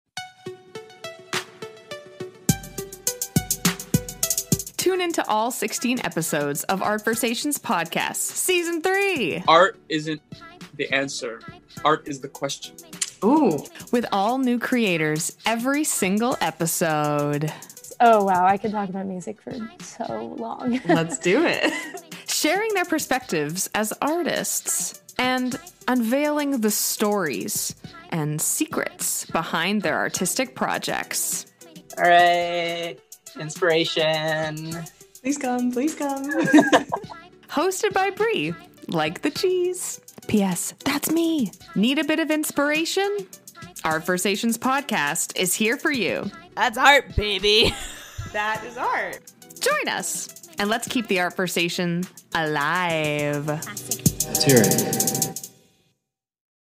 (Art)versations Podcast | S3 TRAILER
Artists. Conversations. (Art)versations.

(Art)versations is a podcast, hosted by Bri Clarke. Each episode, a creator will sit down to discuss art in any and all of its forms. Season 3 takes an in-depth look at how COVID-19 has affected the performance industry. We chat, we laugh, and we share perspectives on artistic projects. Let's save the arts!
(Art)versations Podcast - Hosted by Bri Clarke

FOLLOW THE PODCAST INSTAGRAM: @artversationspod
FOLLOW BRI’S INSTAGRAM: @brianna_clarkee

Photo credits (from top left): The Playground Project by Ashlyn Abbott; Alessia Urbani by Sydney Grogan; Ricardo Montilla Lopez by Kim Nguyen-Coleman; Aaliyah Sundar by Kaitlyn Goss; Michael Baboolal by Olivia Leblanc; Laith Al-Kinani by Elena Emer; Claire DeJean by Jordan Puhala; Madison Leonardo by Christian Lavigne; Samuel Davilmar by David Leyes; Sadie Segriff by Gabrielle Tyrie; Sierra Holder by Emily Battaglini; Rohan Dhupar by Sam Gaetz; Melanie Elysse Old by Alvin Collantes; Wesley Tang by Julian Elia; Colleen Snell by Noelle Hamlyn; Ellen Winter by Alison Grasso and Colin Miller.